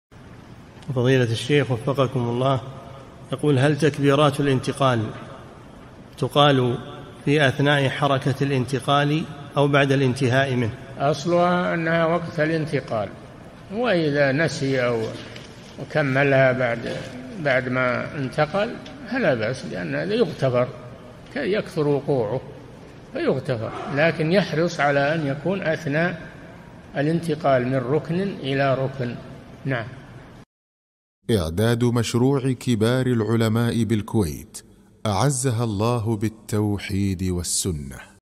[313 -582] هل تكبيرات الانتقال تقال في أثناء حركة الانتقال أو بعد الانتهاء منه؟ - الشيخ صالح الفوزان
-"السؤال: يقول: هل تكبيرات الانتقال تقال في أثناء حركة الانتقال أو بعد الانتهاء منه؟
الجَوَابُ: أصلها أنها وقت الانتقال وإذا نسي وكملها بعدما انتقل فلا بأس؛ لأن هذا يغتفر، يكثر وقوعه فيغتفر لكن يحرص على أن يكون أثناء الانتقال من ركن إلى ركن.
للاشتراك في قناة الشيخ صالح الفوزان
سؤال وجواب من شرح صفة الصلاة ( 582 ) مقطع

للاستفسار والإقتراحات والدعم
رئيس الفريق
لمشروع كبار العلماء